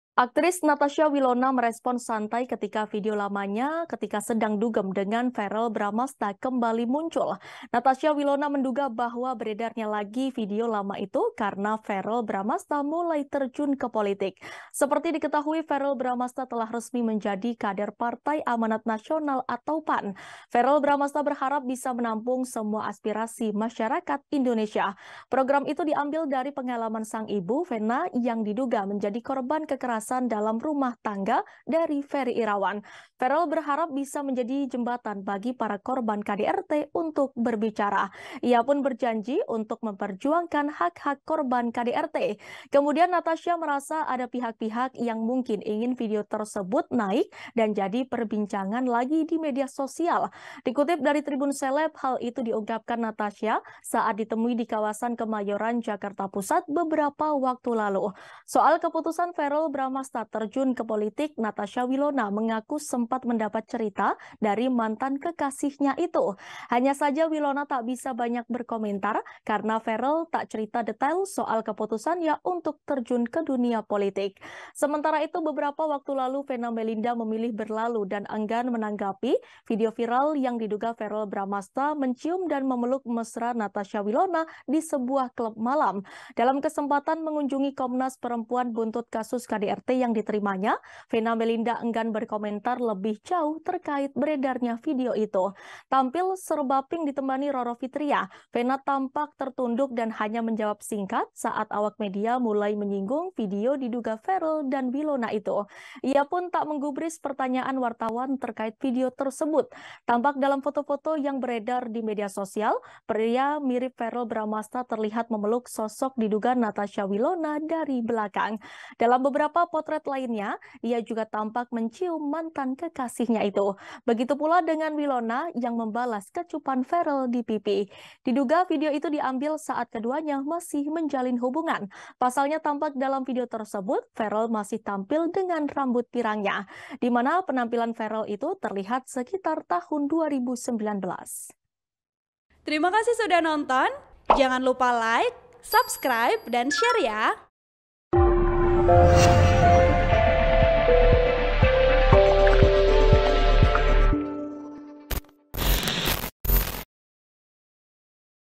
Video saat Dugem dengan Verrel Bramasta Viral, Natasha Wilona Tanggapi Santai: Mau Gimana Lagi
TRIBUN-VIDEO.COM - Aktris Natasha Wilona merespon santai ketika video lamanya ketika sedang dugem dengan Verrell Bramasta kembali muncul.

Natasha Wilona menduga bahwa beredarnya lagi video lama itu karena Verrell Bramasta mulai terjun ke politik.

Seperti diketahui, Verrell Bramasta telah resmi menjadi kader Partai Amanat Nasional (PAN).

Verrell Bramasta berharap bisa menampung semua aspirasi masyarakat Indonesia.

Program itu diambil dari pengalaman sang ibu, Venna yang diduga menjadi korban kekerasan dalam rumah tangga (KDRT) dari Ferry Irawan.

Verrell berharap bisa menjadi jembatan bagi para korban KDRT untuk berbicara.

Ia pun berjanji untuk memperjuangkan hak-hak korban KDRT.

Dikutip dari Tribun Seleb, hal itu diungkapkan Natasha saat ditemui ddi kawasan kemayoran, Jakarta Pusat beberapa waktu lalu.

Soal keputusan Verrell Bramasta terjun ke politik, Natasha Wilona mengaku sempat mendapat cerita dari mantan kekasihnya itu.

Hanya saja Wilona tak bisa banyak berkomentar karena Verrell tak cerita detail soal keputusannya untuk terjun ke dunia politik.

Sementara itu, beberapa waktu lalu Venna Melinda memilih berlalu dan enggan menanggapi video viral yang diduga Verrell Bramasta mencium dan memeluk mesra Natasha Wilona di sebuah klub malam.

Dalam kesempatan mengunjungi Komnas Perempuan buntut kasus KDRT yang diterimanya, Venna Melinda enggan berkomentar lebih jauh terkait beredarnya video itu.

Tampil serba pink ditemani Roro Fitria, Venna tampak tertunduk dan hanya menjawab singkat saat awak media mulai menyinggunh video diduga Verrell dan Wilona itu.

Ia pun tak menggubris pertanyaam wartawan terkait video tersebut.

Dalam beberapa potret lainnya, ia juga tampak mencium mantan kekasihnya itu.

Begitu pula dengan Wilona yang membalas kecupan Verrell di pipi.

Diduga video itu diambil saat keduanya masih menjalin hubungan.

Pasalnya, tampak dalam video tersebut, Verrell masih tampil dengan rambut pirangnya.

Di mana penampilan Verrel itu terlihat sekitar tahun 2019.

Artikel ini telah tayang di Tribunnews.com dengan judul Video Lamanya Dugem dengan Verrell Bramasta Beredar Lagi, Natasha Wilona Menduga Ada Urusan Politik,

Video Production: Abdul Salim Maula Safari Thoyyib